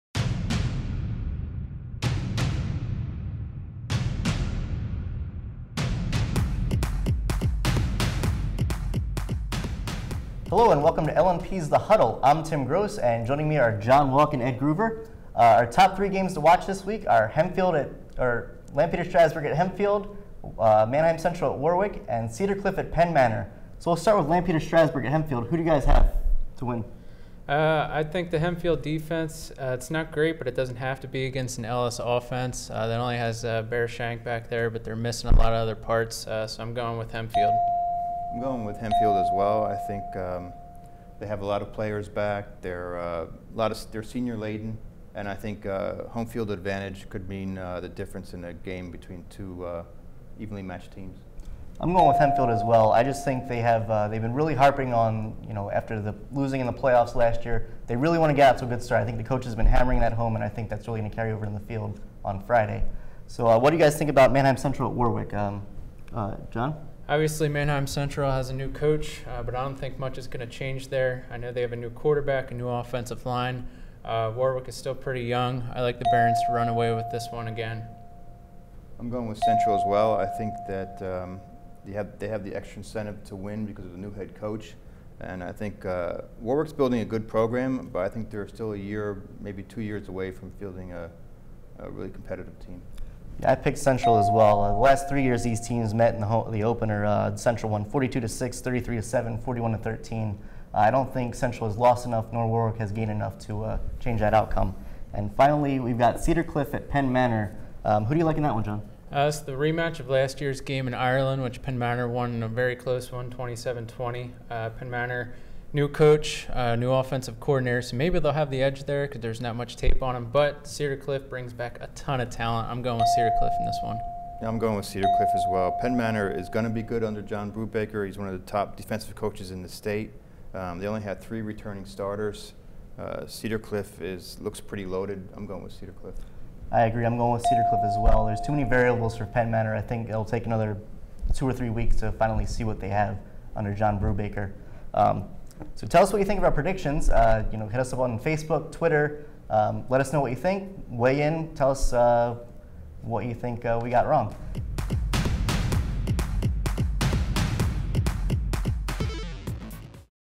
The Huddle: Week 1 Friday night football game predictions
LNP sports reporters Tim Gross, John Walk and Ed Gruver share their game predictions for three of Friday night's L-L league games: Lampeter-Strasburg at Hempfield, Manheim Central at Warwick, and Cedar Cliff at Penn Manor.